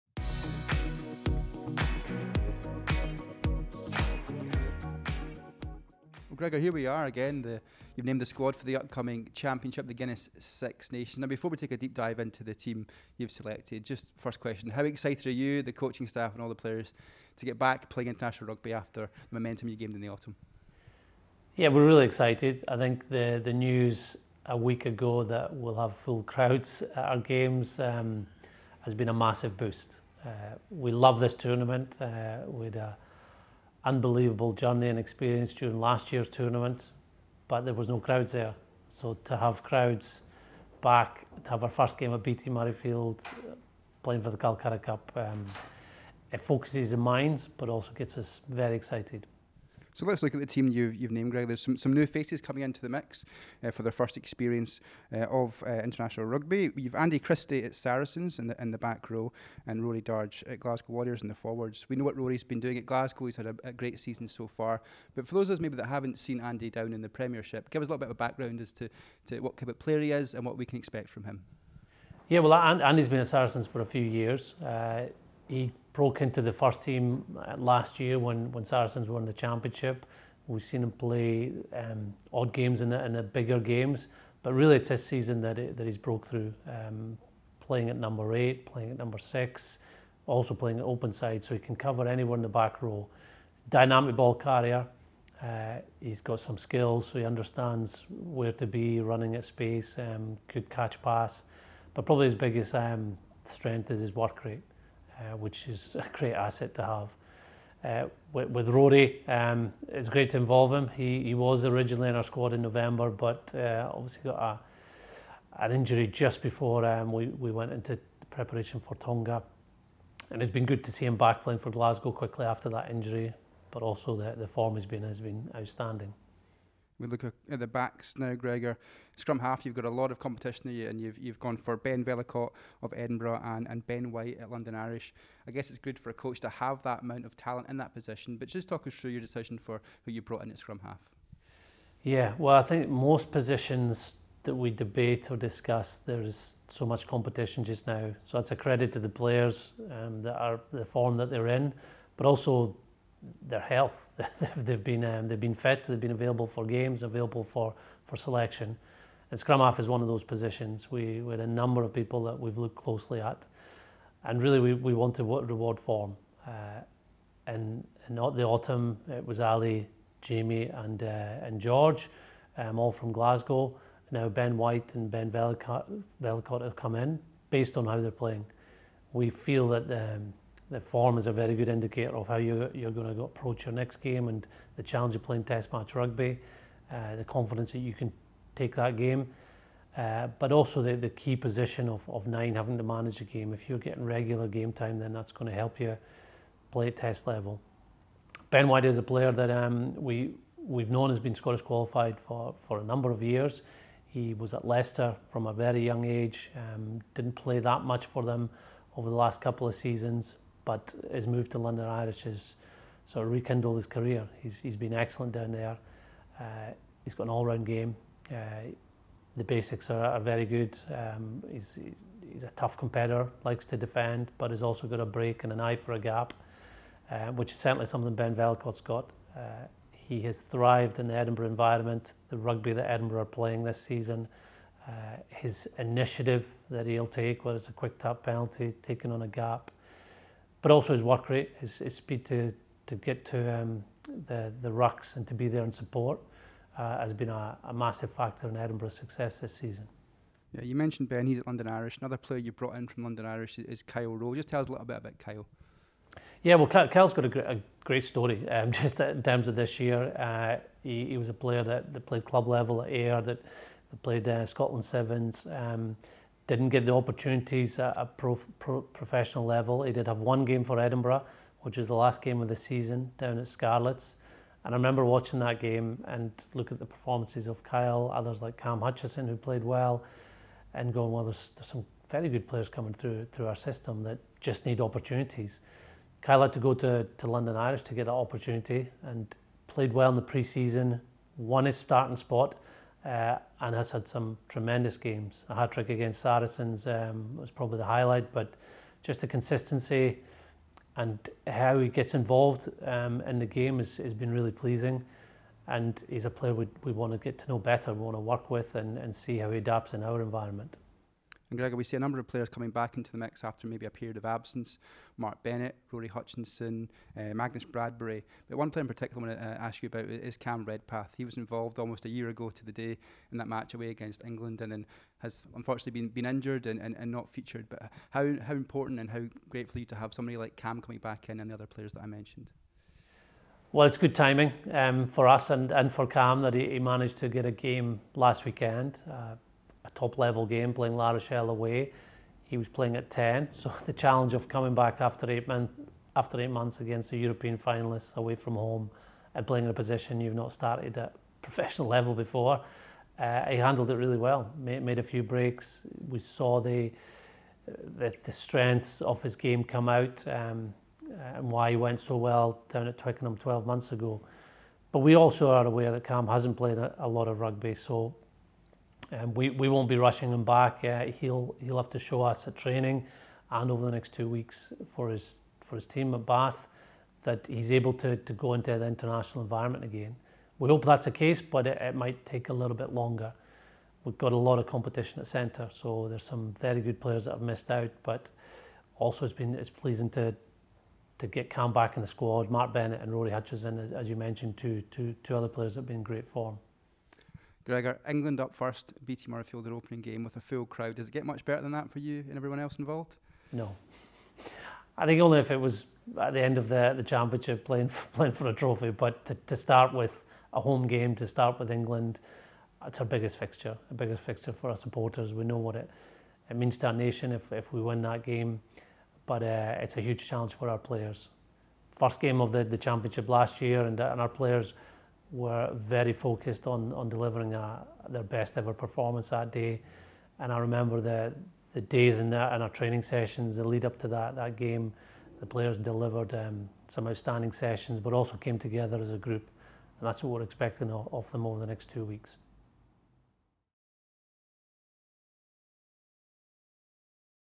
Gregor Townsend names Scotland squad for 2022 Guinness Six Nations
Scotland Head Coach Gregor Townsend talks about the 39-man squad he has named for the upcoming Guinness Six Nations